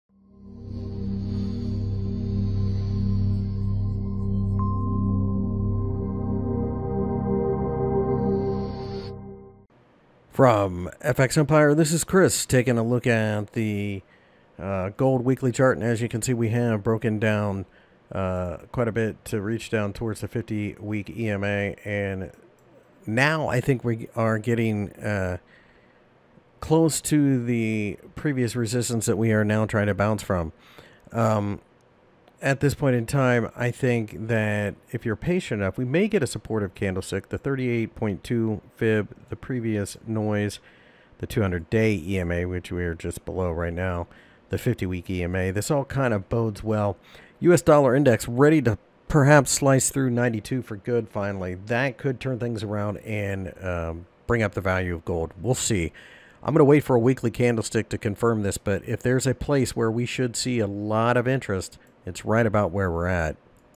Gold Technical Analysis for the Week of November 30, 2020 by FXEmpire
Gold markets got hammered during the course of the week as we continue to see a lot of money flow into the “risk on” trade. With this being the case, gold was ditched and fell below the $1800 level.

Find more information about Forex and Commodities News Technical Analysis Fundamental Analysis and Brokers on FX Empire